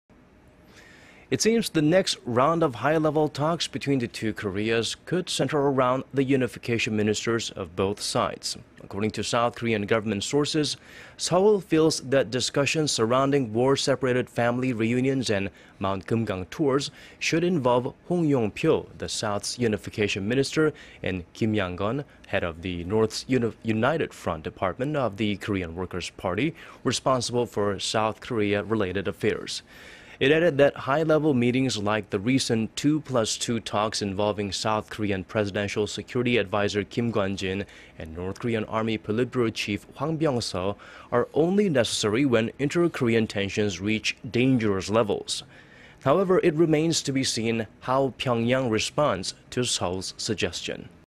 Two Koreas could arrange talks for unification ministers of both sides 남측 통일부장
It seems the next round of high level talks between the two Koreas could center around the unification ministers of both sides.
According to South Korean government sources， Seoul feels that discussions surrounding war－separated family reunions and Mount Kumgang tours should involve Hong Yong－pyo， the South′s Unification Minister and Kim Yang－gon...， head of the North′s United Front Department of the Korean Workers′ Party， responsible for South Korea related affairs.
It added that high－level meetings， like the recent 2 plus 2 talks involving South Korean presidential security adviser Kim Kwan－jin and North Korean Army politburo chief Hwang Pyong－so，... are only necessary when inter－Korean tensions reach dangerous levels.
However， it remains to be seen how Pyongyang responds to Seoul′s suggestion.